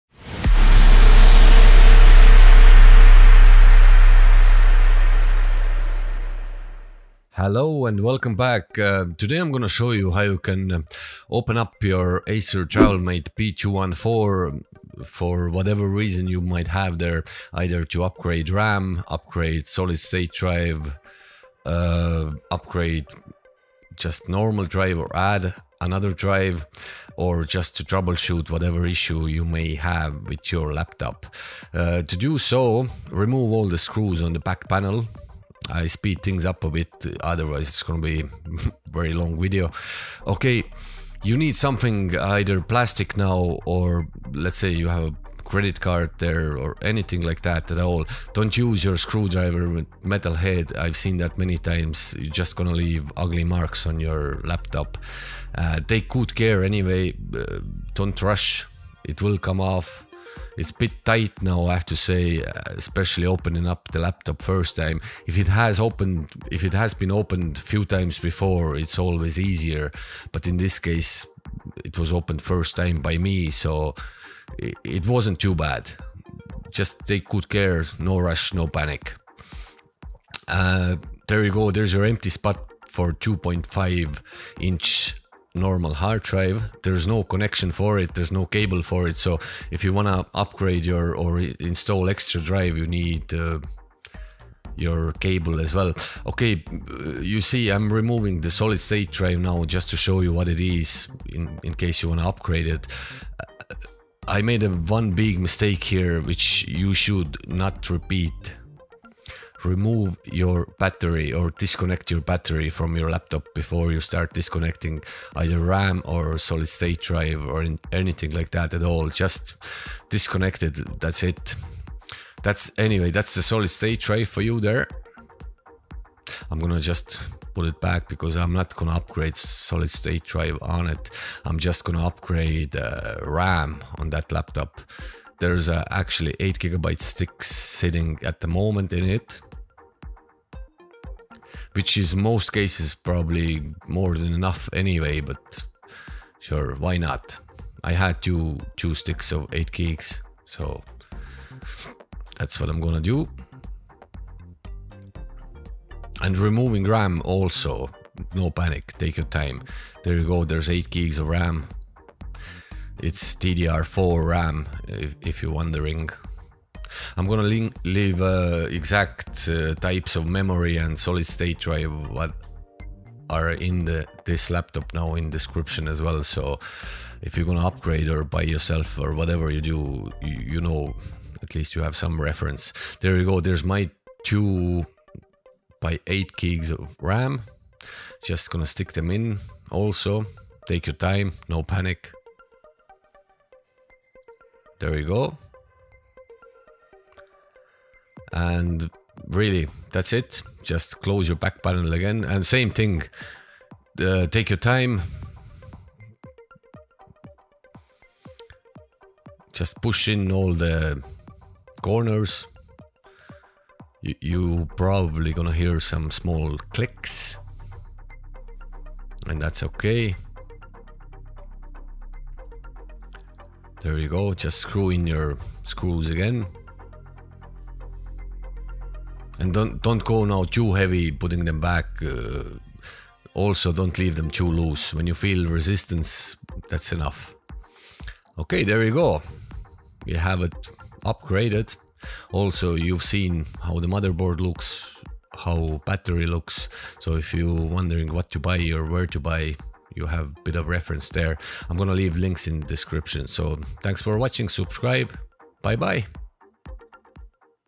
Acer Travelmate P214-52 - How to open and upgrade RAM/SSD/HDD
Here is simple and fast upgrade to your Acer
RAM what was originally in it: 8GB 1RX8 PC4-2666V-SA1-11 (DDR4)
UPGRADED TO: 2 X 8GB 1RX8 PC4-2400T-SA1-11 (DDR4)

SSD in it:  Kingston 128GB SSD M.2 2280 RBU-SNS8154P3/128GJ

CHAPTERS

00:00 Intro
00:11 How to open
01:20 Components
01:30 Upgrading SSD
02:15 RAM Upgrade
03:20 Close back panel